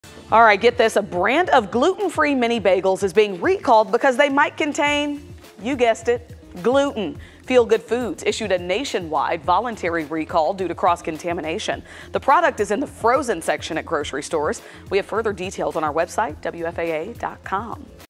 Feel Good Foods gluten-free mini bagels recalled for possibly having gluten
The recall is for Cream Cheese Stuffed Plain Mini Bagels and Cream Cheese Stuffed Everything Mini Bagels with Best Buy dates 5/29/24-9/17/25.